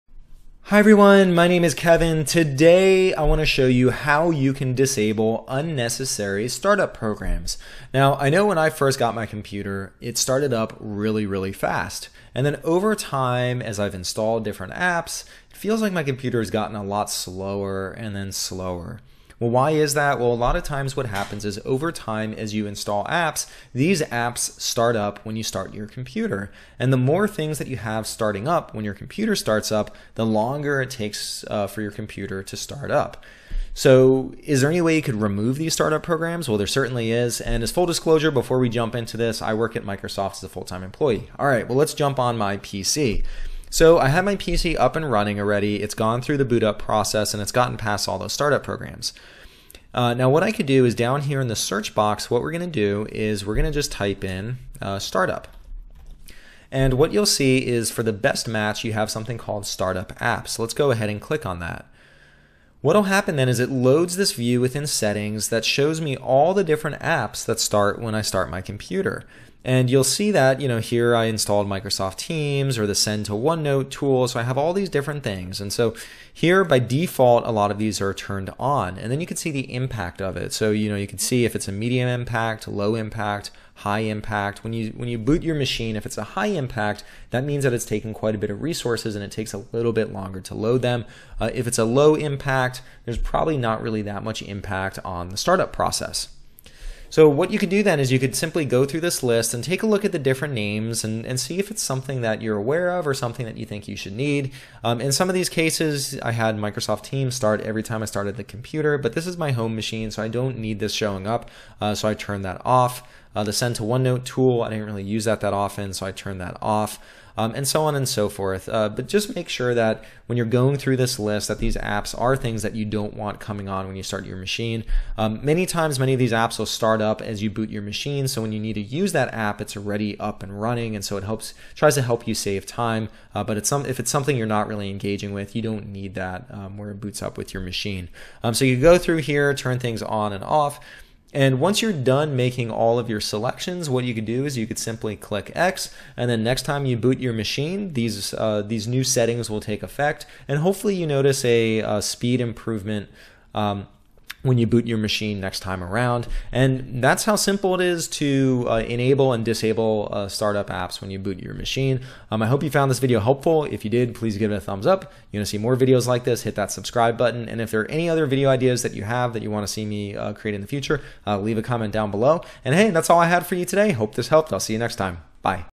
How to Disable Startup Programs in Windows 10
Learn how to quickly and easily disable and change what programs start up when you start Windows 10. Is your PC going slow? Start up programs can affect the time that it takes your PC to boot up. By disabling apps that you don't use, you can improve your PC boot time and performance.

As full-disclosure, I work at Microsoft as a full-time employee.